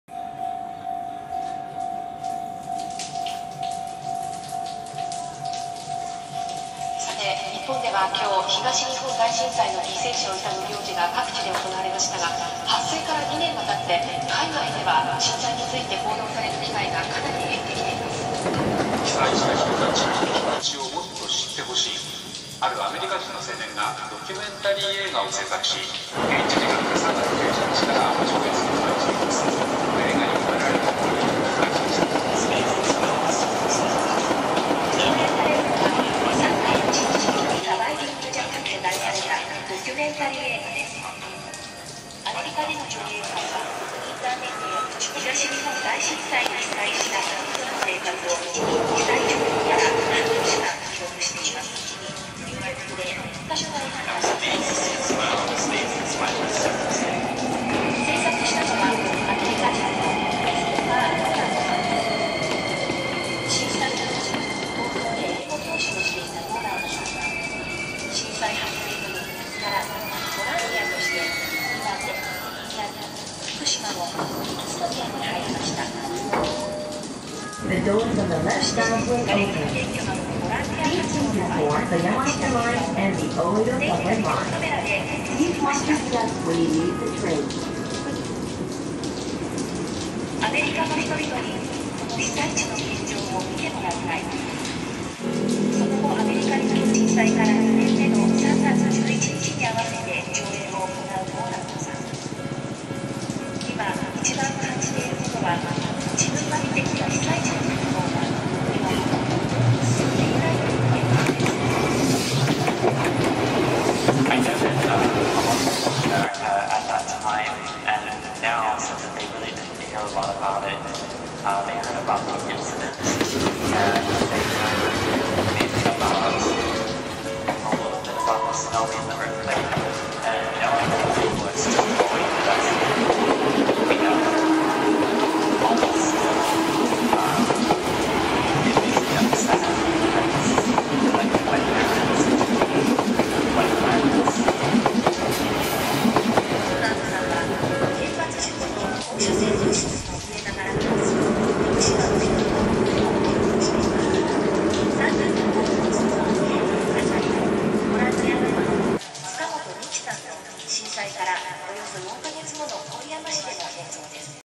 HELP NEEDED : "The Lonely train Goes On as the Children of Fukushima rail toward darkness"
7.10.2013 News from many: Help children of Fukushima... demand evacuation of young one's. Inform other humans about true facts of  what is happening in Japan!
Gravity of situation IS Worldwide ALARM. Thank You.
Published on Oct 7, 2013 Survivingjapanmovie.com

Our thanks go to Chris Nolan for sharing this significant art intervention.
Namaste